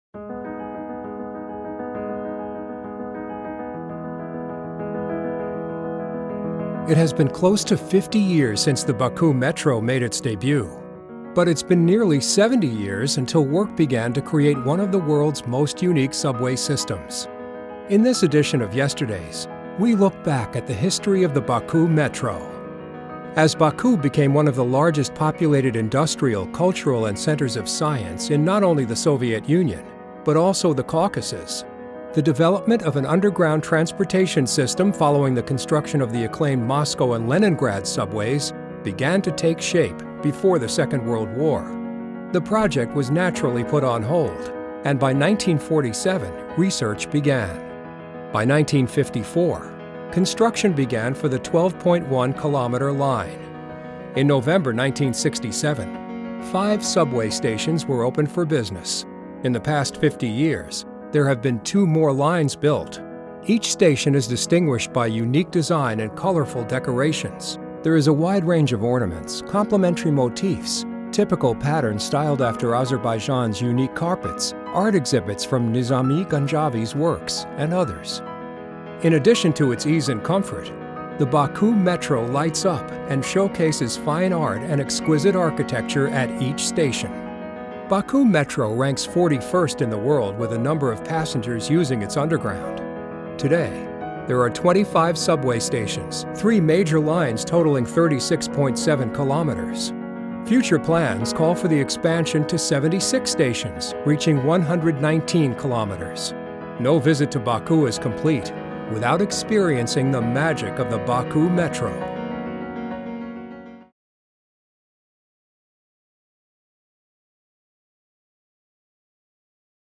Baku Metro
On the 50th anniversary of the Baku Metro, Visions takes a fond look back at the development of one of the world’s lesser-explored subway systems.

Also see the Visions of Azerbaijan magazine article about the Baku Metro